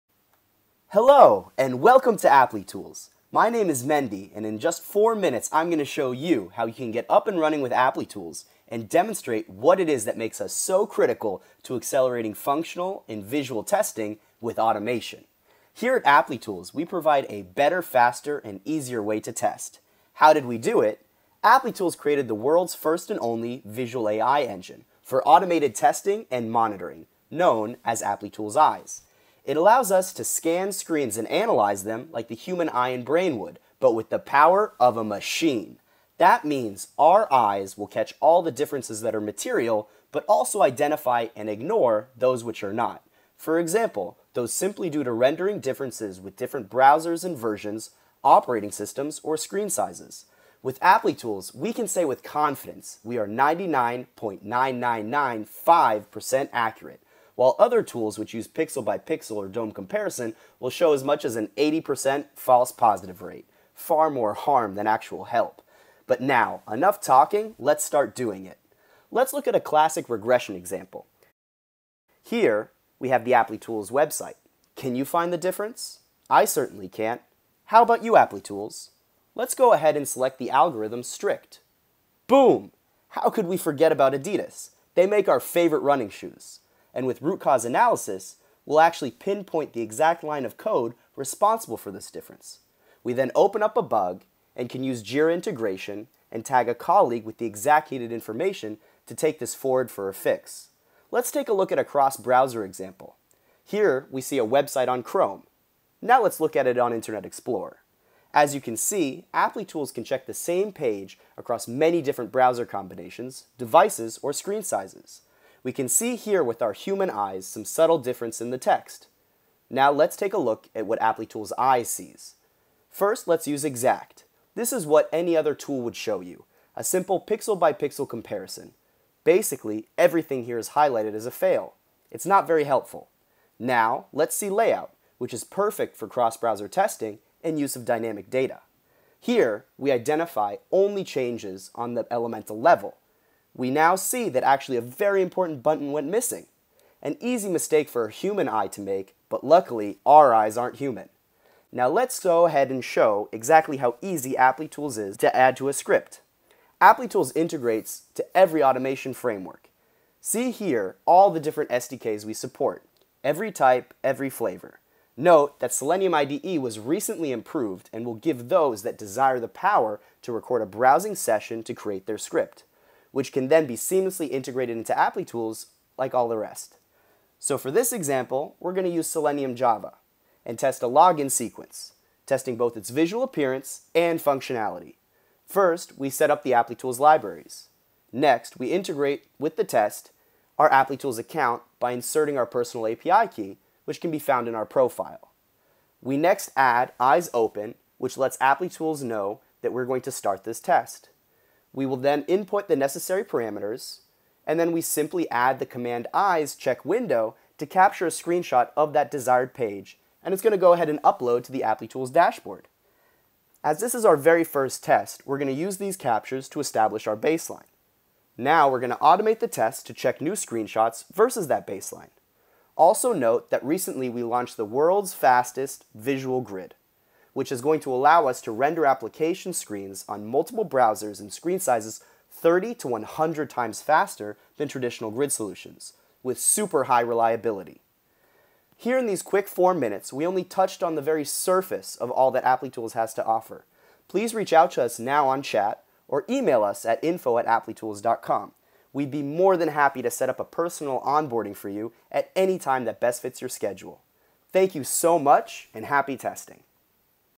What is Applitools Eyes?
Applitools lets you deliver visually perfect web and mobile apps with AI-powered visual testing.

[00:00:00] Hello and welcome to Applitools. My name is Mendy. And in just four minutes I'm going to show you how you can get up and running with Applitools and demonstrate what it is that makes us so critical to accelerating functional and visual testing with automation.

[00:00:15] Here at Applitools we provide a better faster and easier way to test. How did we do it? Applitools created the world's first and only visual A.I. engine for automated testing and monitoring known as Applitools Eyes. It allows us to scan screens and analyze them like the human eye and brain would, but with the power of a machine. That means our eyes will catch all the differences that are material but also identify and ignore those which are not. For example those simply due to rendering differences with different browsers and versions, operating systems, or screen sizes. With Applitools. We can say with confidence we are 99.9995% accurate, while other tools which use pixel by pixel, or dumb comparison, will show as much as an 80% false positive rate - far more harm than actual help. But now, enough talking let's start doing it. Let's look at a classic regression example.

[00:01:12] Here we have the Applitools web site. Can you find the difference? I certainly can't. How about you, Applitools? Let's go ahead and select the algorithm, 'strict'. Boom! How could we forget about Adidas? They make our favorite running shoes. And with Root Cause Analysis, we'll actually pinpoint the exact line of code responsible for this difference. We then open up a bug and can use Jira integration and tag a colleague with the exact needed information to take this forward for a fix.

[00:01:41] Let's take a look at a cross browser example. Here we see a website on Chrome. Now let's look at it on Internet Explorer. As you can see, Applitools can check the same page across many different browser combinations, devices, or screen sizes. We can see here with our human eyes some subtle difference in the text. Now let's take a look at what Applitools Eyes sees. First let's use "Exact". This is what any other tool would show you. A simple pixel by pixel comparison. Basically, everything here is highlighted as a fail. It's not very helpful. Now let's see "Layout", which is perfect for cross browser testing and use of dynamic data. Here, we identify only changes on the elemental level. We now see that actually a very important button went missing. An easy mistake for a human eye to make but luckily our eyes aren't human. Now let's go ahead and show exactly how easy Applitools is to add to a script. Applitools integrates to every automation framework. See here all the different SDKs we support - every type, every flavor. Note that Selenium IDE was recently improved and will give those that desire the power to record a browsing session to create their script, which can then be seamlessly integrated into Applitools, like all the rest.

[00:02:57] So for this example we're gonna use Selenium Java and test a login sequence, testing both its visual appearance and functionality. First, we set up the Applitools libraries. Next, we integrate with the test our Applitools account by inserting our personal API key, which can be found in our profile. We next add "eyes.open", which lets Applitools know that we're going to start this test. We will then input the necessary parameters and then we simply add the command "eyes.checkWindow" to capture a screenshot of that desired page and it's gonna go ahead and upload to the Applitools dashboard. As this is our very first test, we're gonna use these captures to establish our baseline. Now we're going to automate the test to check new screenshots versus that baseline. Also, note that recently we launched the world's fastest visual grid, which is going to allow us to render application screens on multiple browsers and screen sizes 30 to 100 times faster than traditional grid solutions with super high reliability.

[00:03:58] Here in these quick four minutes, we only touched on the very surface of all that Applitools has to offer. Please reach out to us now on chat, or email us at . We'd be more than happy to set up a personal onboarding for you at any time that best fits your schedule. Thank you so much, and happy testing.